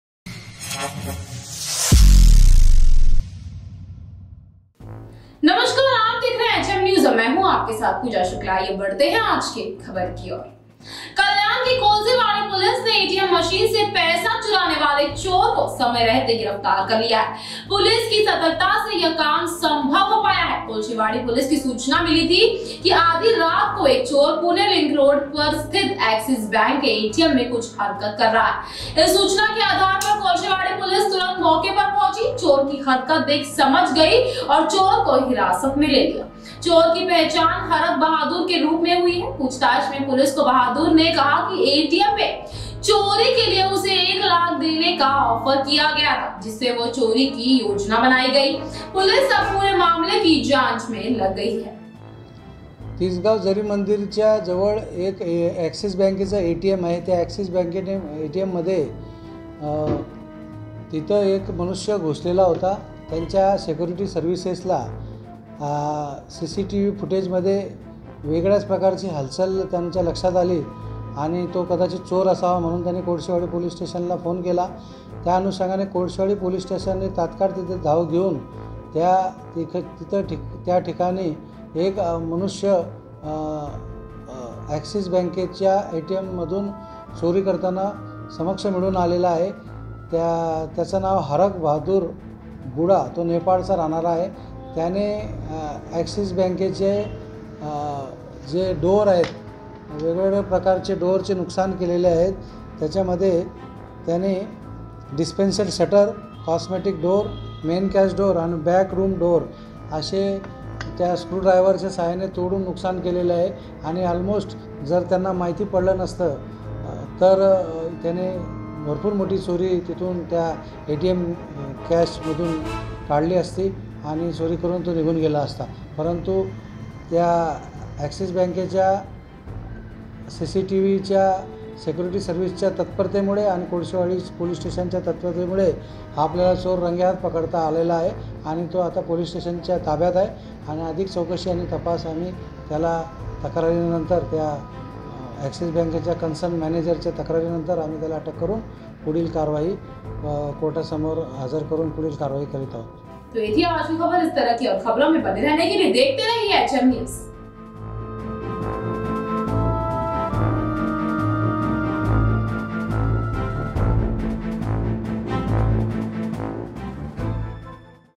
KALYAN: पुलिस की सतर्कता से ATM चोर रंगे हाथ हुआ गिरफ्तार - HM News
The Kolsevadi police of Kalyan has arrested the thief who stole money from the ATM machine in time. This work has been possible due to the vigilance of the police. The Kolsevadi police had received information that at midnight a thief was doing some movement in the ATM of Axis Bank located on Pune Link Road.